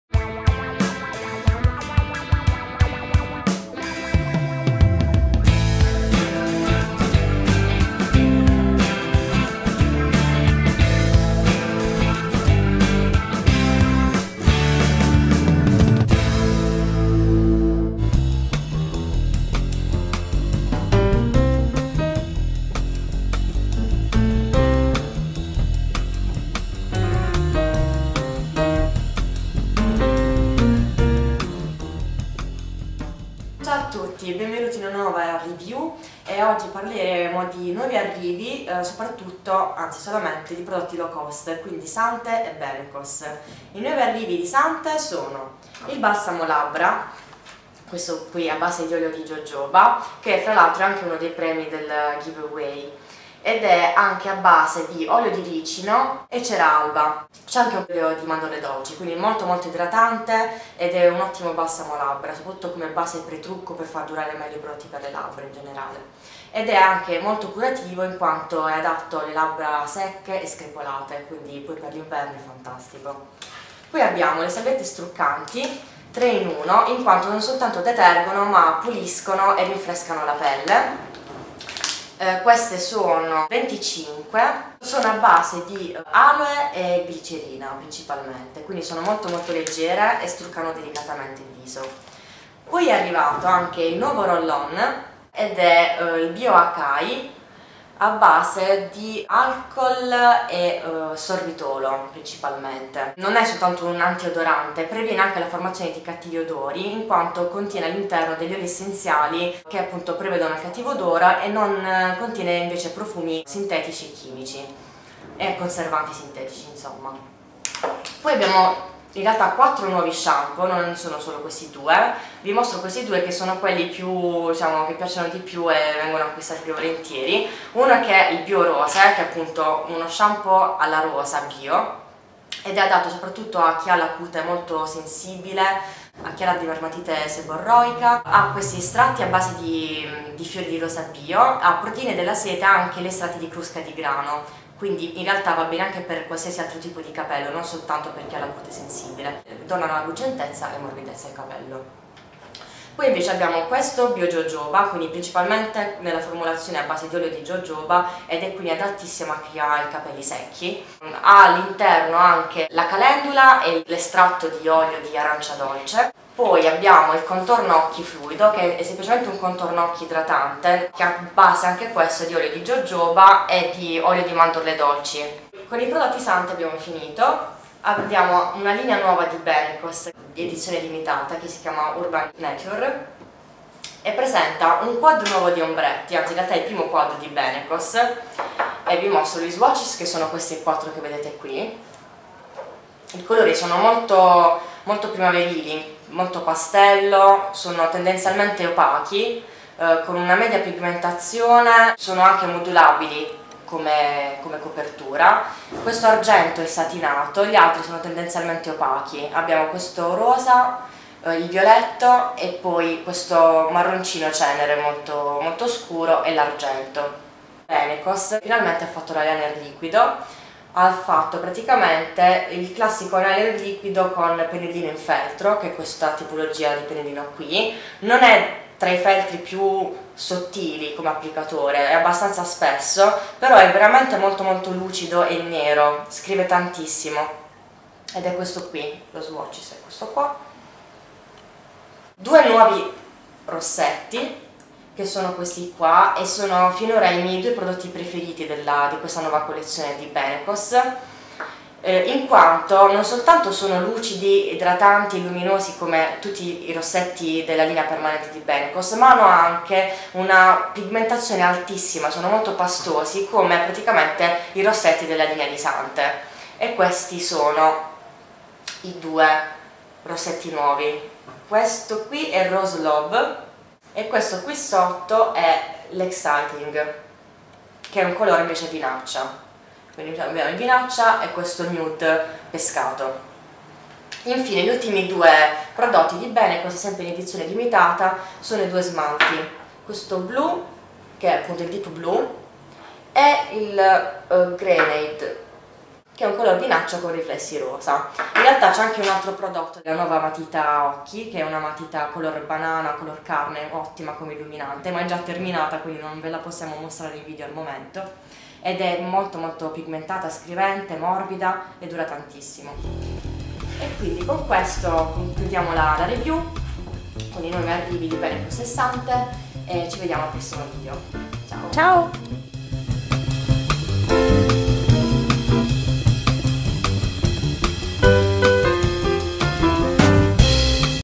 Nuovi arrivi low cost: Sante & Benecos!
I PRODOTTI CITATI:

SANTE:

Lip Balm Bio-Goji
Salviette Struccanti Bio-Goji
Roll-On Deo Bio-Acai
Shampoo Bio-Rose
Shampoo Bio-Jojoba
Fluido per gli occhi Bio-Acai

BENECOS:

Quad Beautiful Eyes 001
Natural Liquid Eyeliner Black
Natural Lipstick Rose Love
Natural Lipstick Exciting
Nail Polish Deep Blue
Nail Polish Grenade